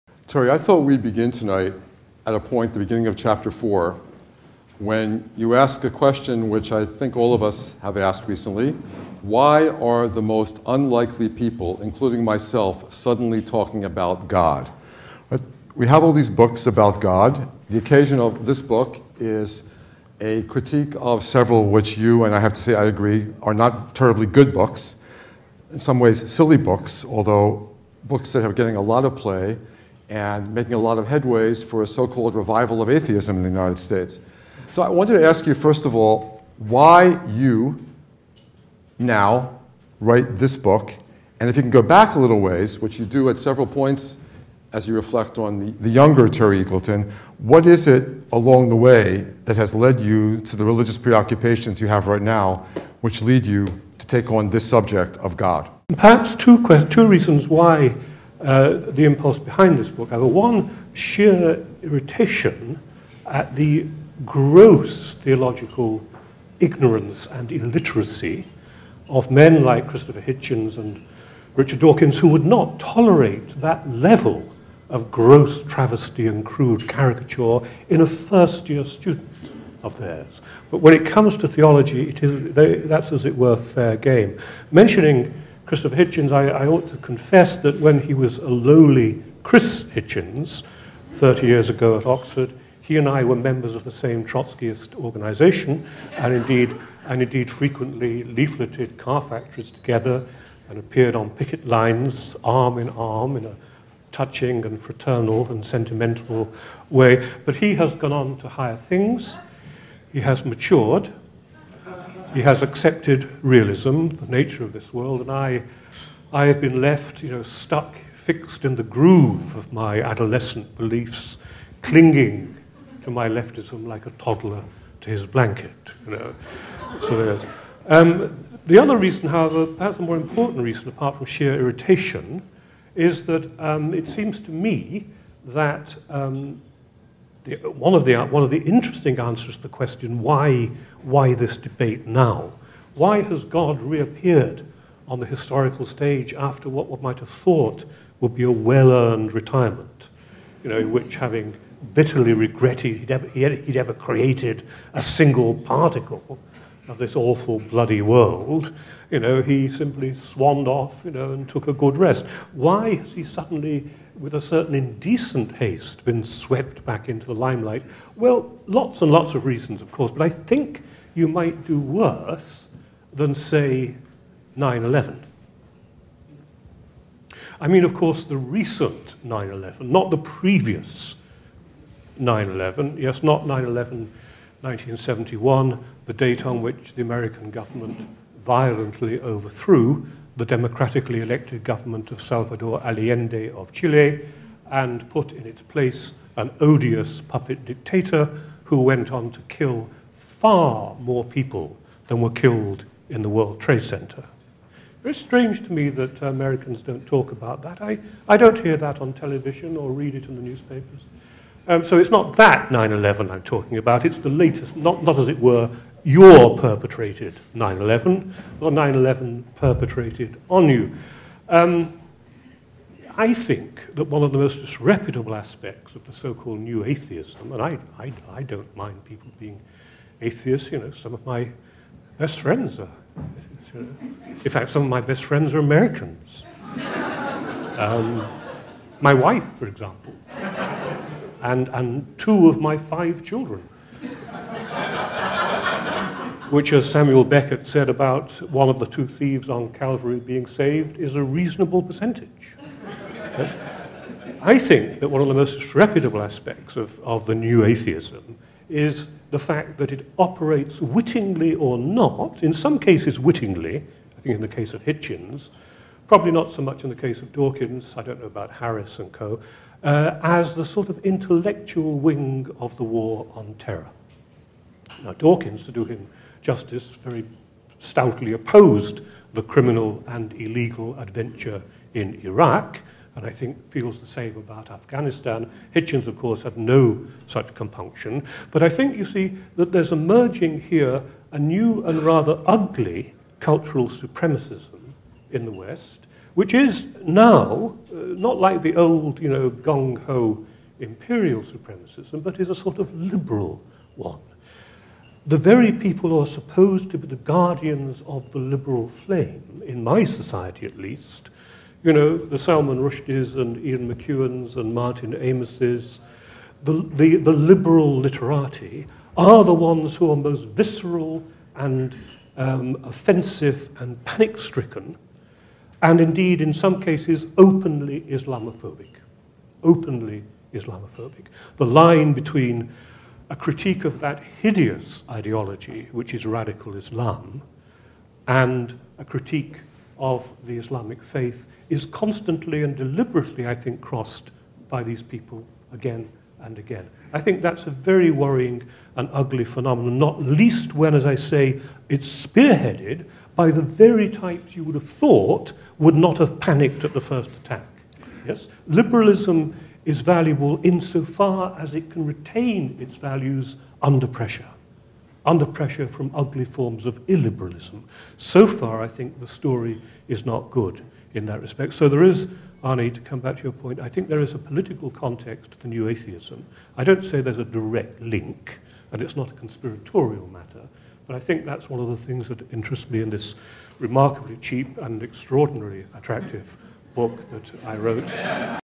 Clip 1: Atheism as Islamophobia (Templeton Foundation)
A conversation between Terry Eagleton, author of "Reason, Faith, and Revolution: Reflections on the God Debate" (Yale University Press), and Arnold Eisen, Chancellor, Jewish Theological Seminary. For more information about Templeton Book Forum events, please

Terry Eagleton's witty and polemical Reason, Faith, and Revolution has caused a stir among scientists, theologians, people of faith, and people of no faith, as well as among general readers eager to understand the current "God debate." Eagleton takes aim at what he calls the "truly shocking ignorance" of religion displayed by Richard Dawkins and Christopher Hitchens in their best-selling atheist manifestos. His own account of the "tragic humanism" at the center of the Western religious tradition includes provocative reflections on death, suffering, and love; on revelation and reasonable belief; on the relationship between science and rationality; and on the peculiarly modern tension between the claims of civilization and culture.

Terry Eagleton is Bailrigg Professor of English Literature at the University of Lancaster, England, and Professor of Cultural Theory at the National University of Ireland, Galway. For the next five years, he will be a Distinguished Visitor for three weeks each semester in the English Department at the University of Notre Dame. A fellow of the British Academy, he is the author of more than forty books, ranging widely over literary theory, cultural studies, religion, politics, and history. He will deliver a Gifford Lecture on the "God Debate" at the University of Edinburgh in March 2010.

Arnold Eisen is the Chancellor of the Jewish Theological Seminary. Before coming to JTS, he was the Koshland Professor of Jewish Culture and Religion at Stanford University. He also served as senior lecturer in the Department of Jewish Philosophy at Tel Aviv University and assistant professor in the Department of Religion at Columbia University. His many publications include a personal essay, Taking Hold of Torah: Jewish Commitment and Community in America; a historical work, Rethinking Modern Judaism: Ritual, Commandment, Community; and (with Steven Cohen) The Jew Within: Self, Family, and Community in America.